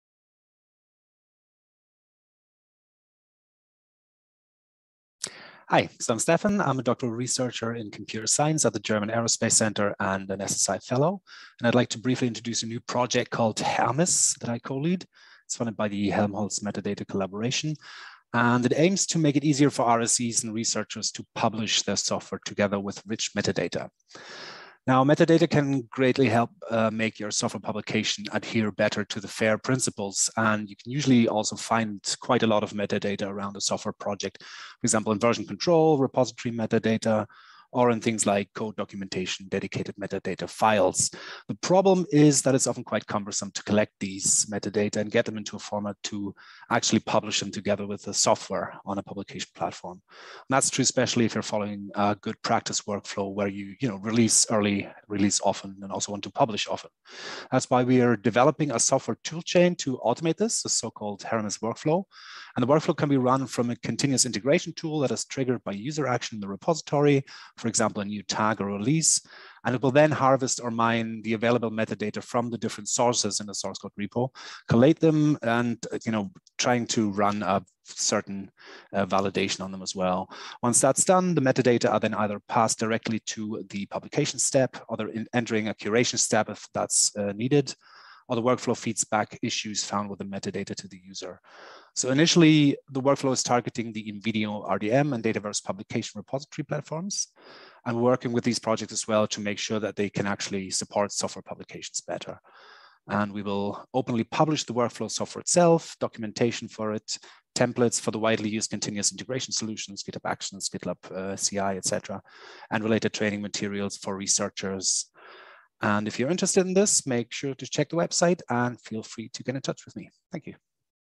Automating software publications for findability, reproducibility & credit - CW22 Lightning Talk
Automating software publications for better findability, reproducibility, and credit by Stephan Druskat on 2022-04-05